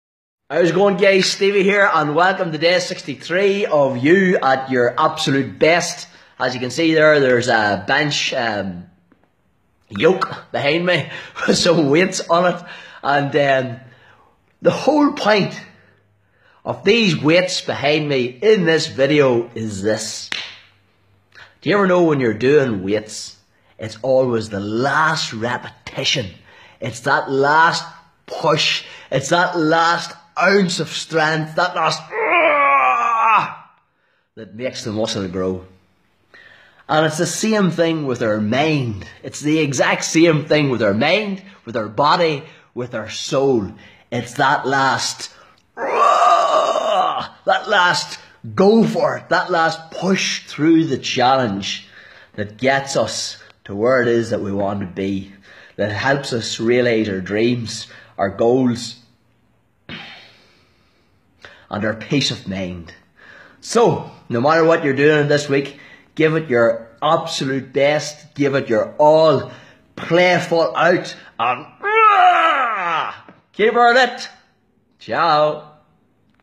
Steveyj life coach: Day 63 being at your absolute best!
Steveyj talks about the last repetition!
Tickets are still available for our next live event begin your own revolution on this